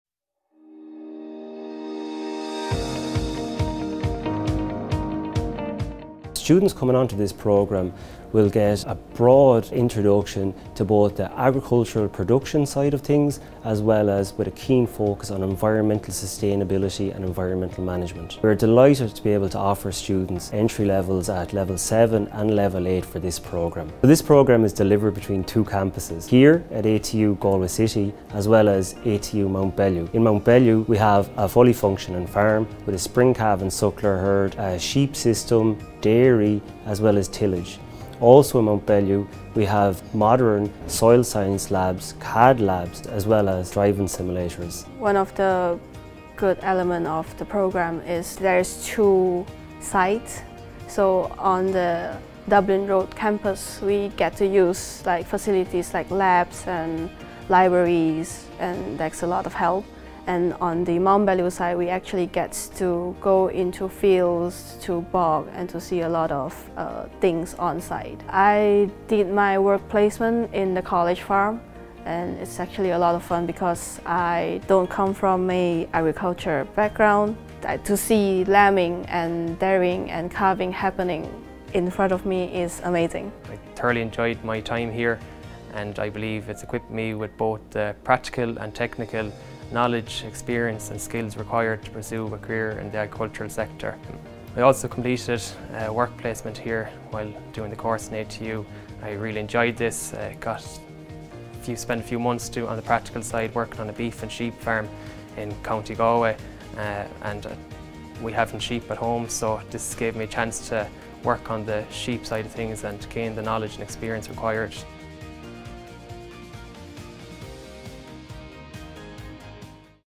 AU657/AU557 - Agriculture and Environmental Management , ATU Mountbellew
AU657 - Level: 8 Bachelor of Science (Honours) in Agriculture and Environmental Management
AU557 - Level: 7 Bachelor of Science in Agriculture and Environmental Management

These programmes provide students with a range of specialised skills ideal for employment in the agriculture and environmental sectors. Strong  emphasis is placed on sustainability in agriculture and the environment. Graduates of the programme can expect to gain employment in a range of areas including farming, agri-foods, environmental management, consultancy, applied ecology and agri-business. These programmes are highly practical, with a mixture of lectures, fieldwork, laboratory practicals and IT.

Hear what Martin McNamara (Lecturer), Jia Xin Chai (Student) and Diarmuid Mulkerrins  (Graduate) had to say about the programme.